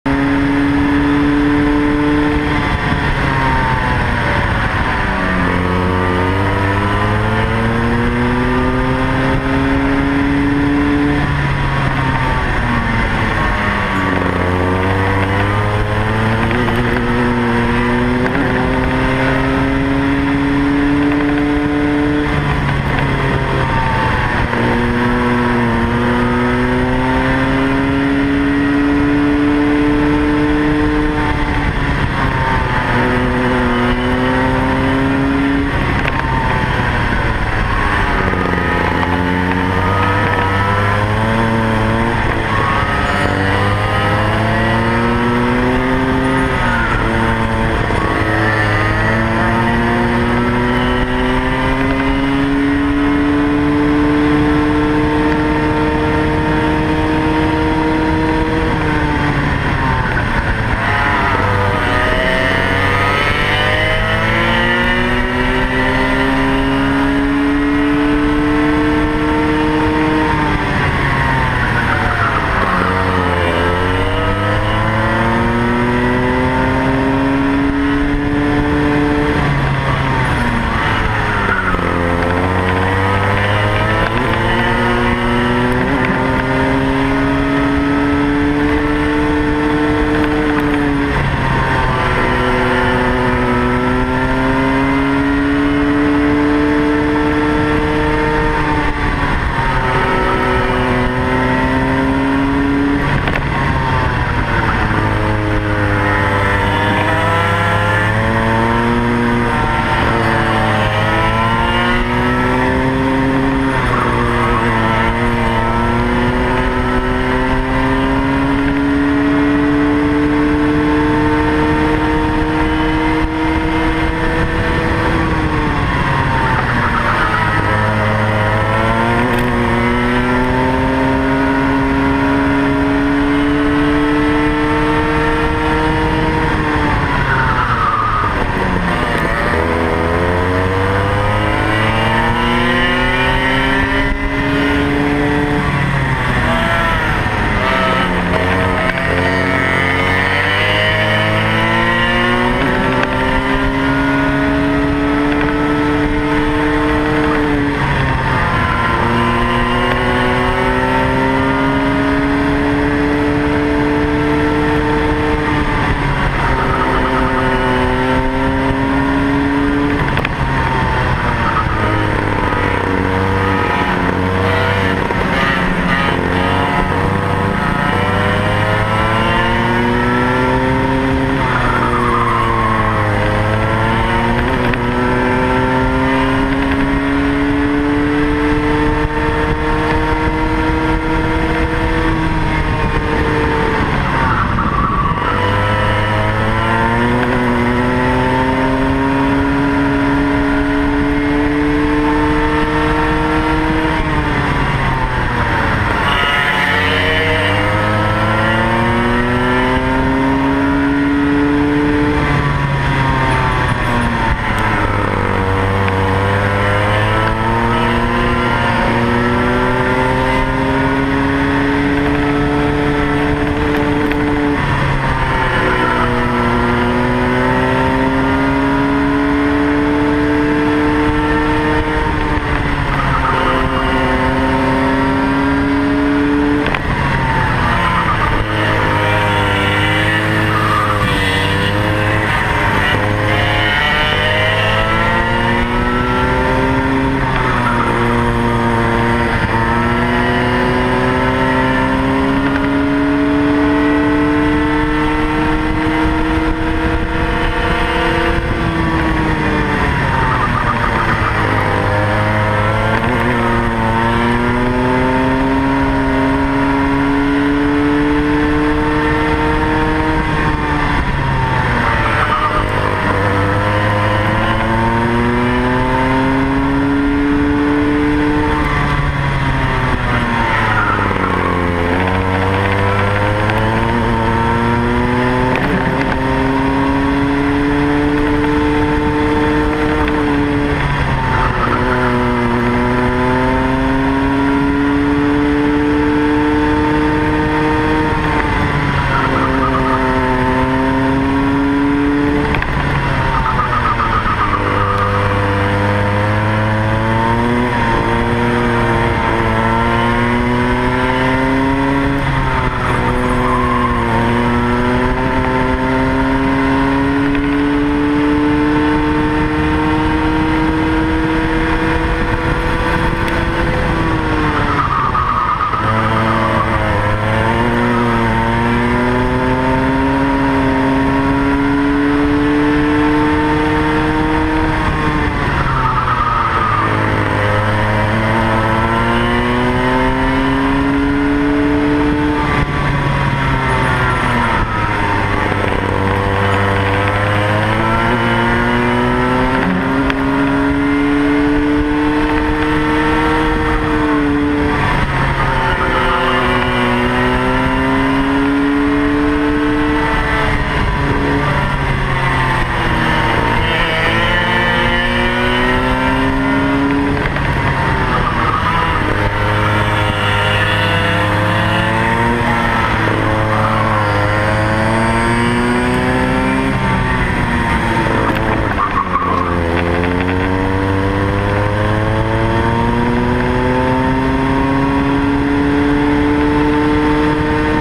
AJR Stogie Practice at Greg Moore Raceways !
ONce again Stogie (LBB) continue his racing career. His goal is to race in the Rotaxmax Challenge in Europe with the best drivers in the world at his age. Stogie is 8 so he has long way to go !

This time, he is practicing with MAX who has a Parilla Comer which is faster than his Rotax Micromax but his driving skills made up the difference.

Thank you to all that has been supported and contibuted their efforts. We will try harder and get ready for the new challenge !